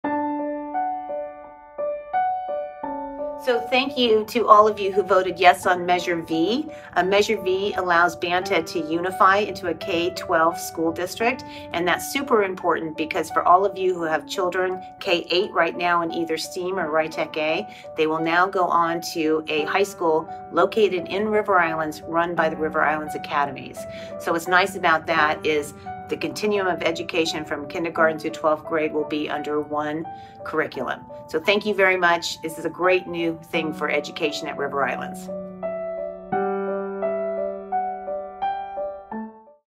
Bringing K-12 to River Islands
River Islands plans to build nine schools within its community, a decision that will greatly accommodate families by eliminating the stress of a long commute, and providing our youngest residents with the full capacity to thrive close to home.